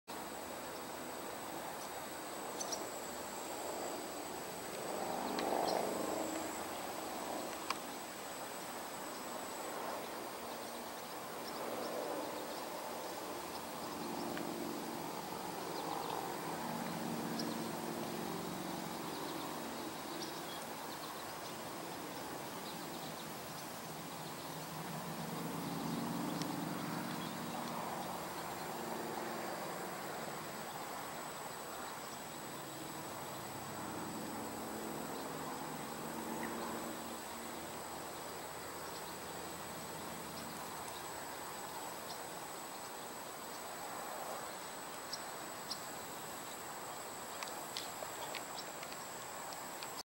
Greater Yellowlegs 1 HQ HD
Profile of some Greater Yellowlegs in a pond.
Filmed at Niagara College Ponds.